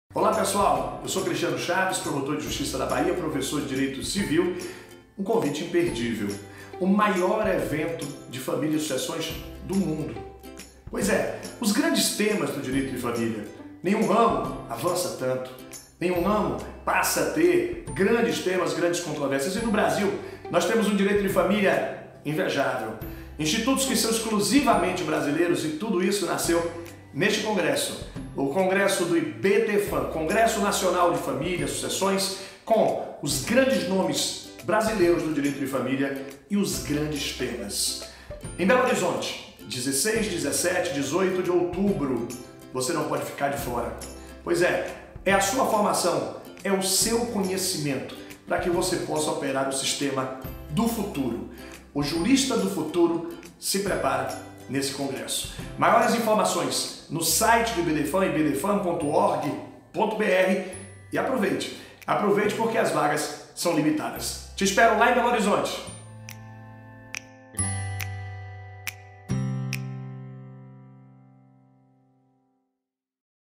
Cristiano Chaves convida
O promotor de Justiça Cristiano Chaves convida para o XII Congresso Brasileiro de Direito das Famílias e Sucessões IBDFAM. Dias 16, 17 e 18 de outubro de 2019, em Belo Horizonte.